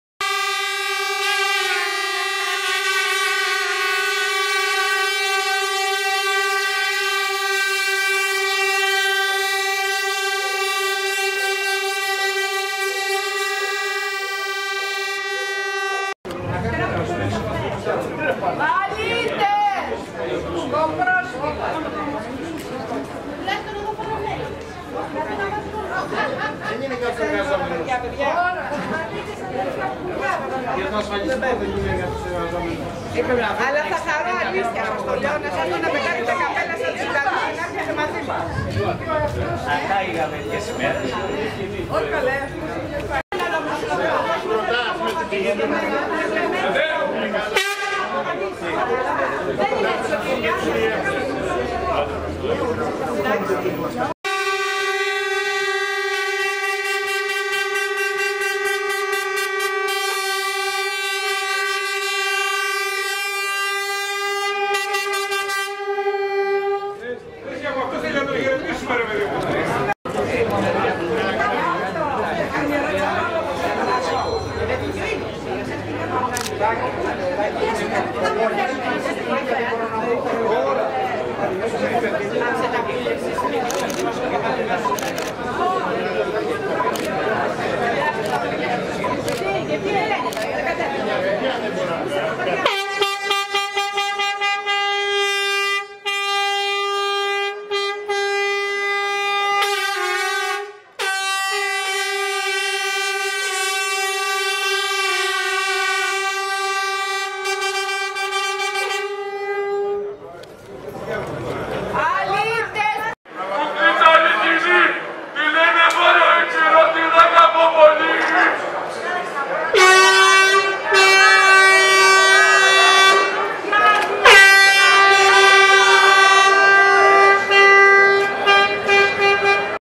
ArgolidaPortal.gr Άργος-Διαμαρτυρία κατά υφυπουργού εργασίας στο Εργατικό Κέντρο
(Βίντεο © eurokinissi-Βασίλης Παπαδόπουλος) Τον υφυπουργό Εργασίας, Κοινωνικής Ασφάλισης και Κοινωνικής Αλληλεγγύης Αναστάσιο Πετρόπουλο που βρέθηκε στο Άργος για προγραμματισμένη ομιλία , υποδέχτηκε με φωνές και συνθήματα κατά της κυβέρνησης ομάδα πολιτών.